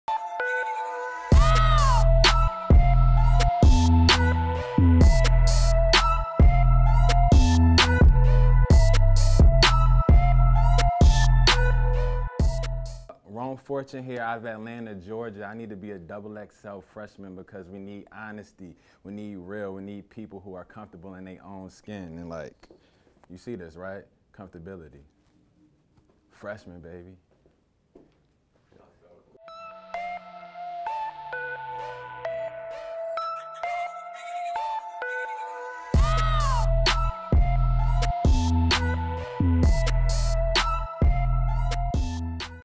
XXL Freshman 2016- Rome Fortune Pitch
XXL Freshman 2016 is coming and the freshman hopefuls are gearing up to possibly make this year’s class. The next pitch comes from Rome Fortune.